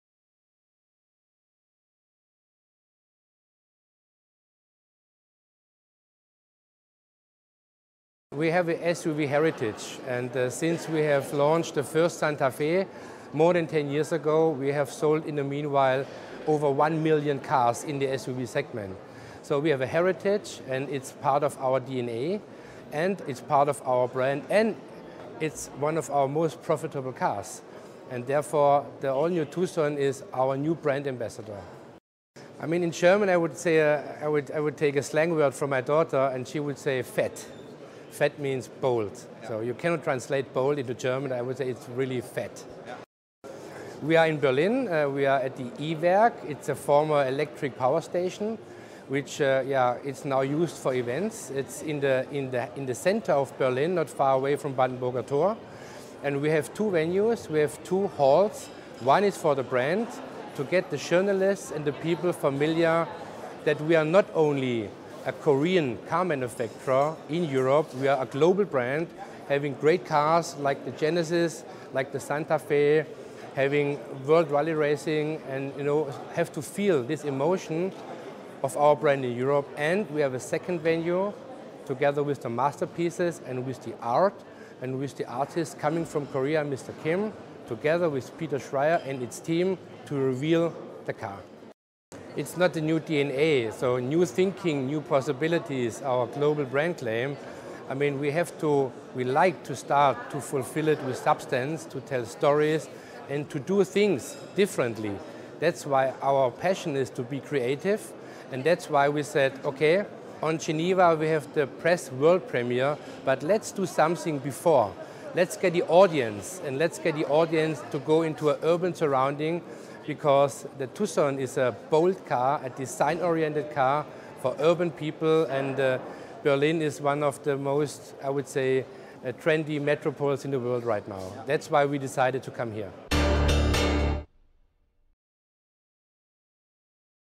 The All-New Hyundai Tucson - Jochen Sengpiehl
Statements - Jochen Sengpiehl, Vice President Marketing at Hyundai Motor Europe

Preview Event of the All-New Hyundai Tucson in Berlin